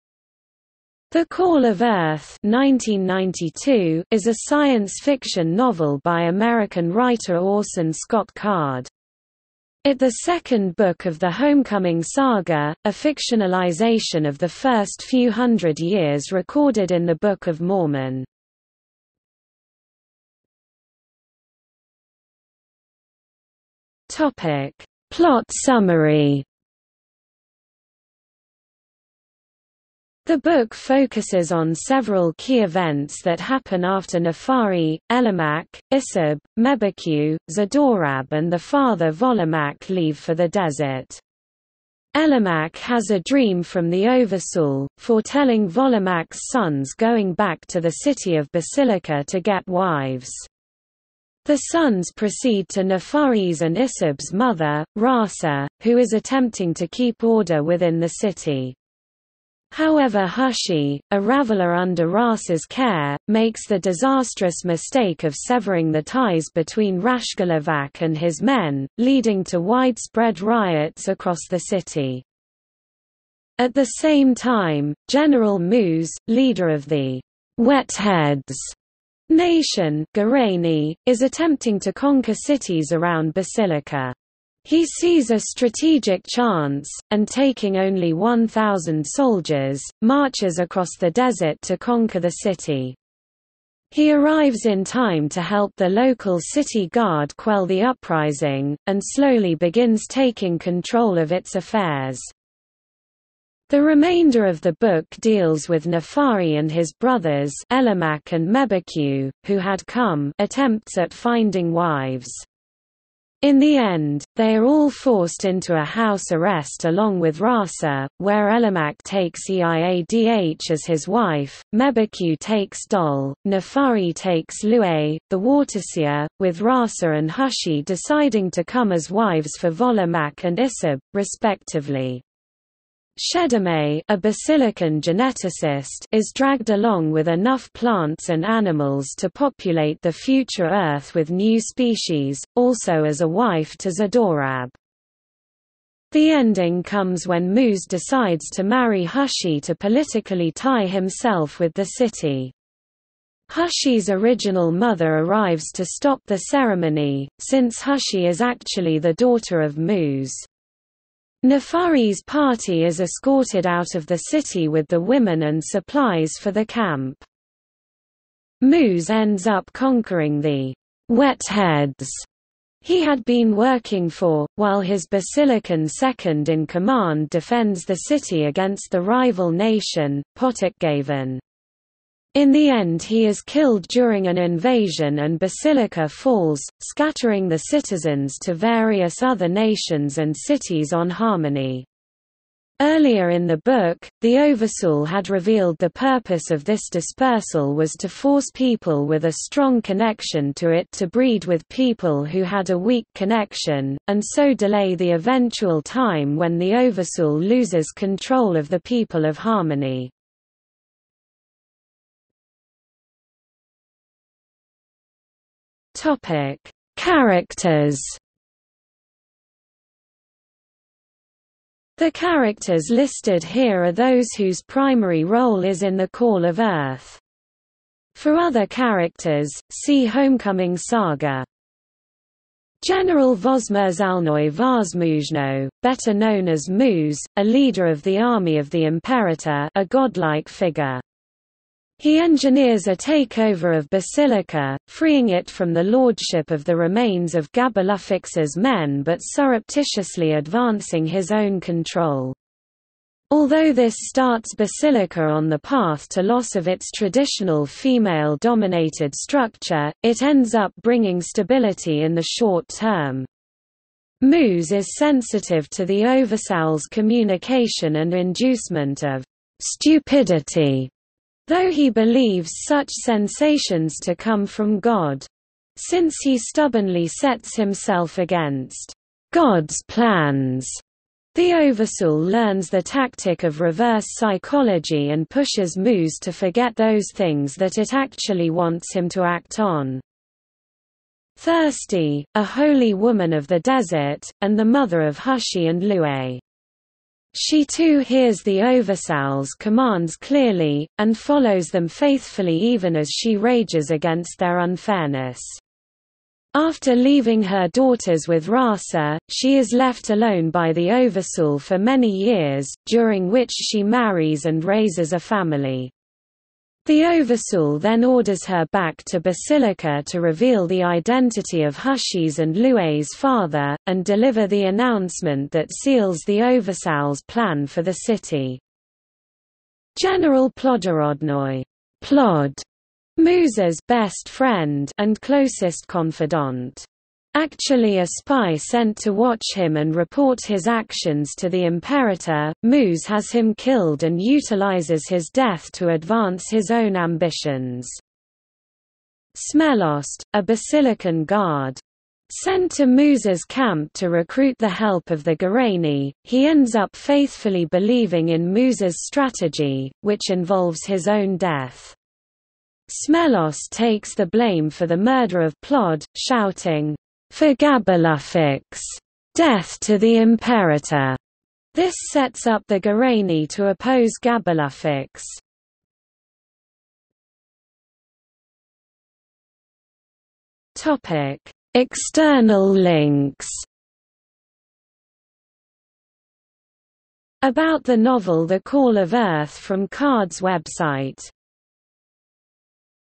The Call of Earth | Wikipedia audio article
00:00:20 Plot summary
00:03:32 Characters
00:06:17 External links

- Socrates

SUMMARY
The Call of Earth (1992) is a science fiction novel by American writer  Orson Scott Card. It  the second book of the Homecoming Saga, a fictionalization of the first few hundred years recorded in the Book of Mormon.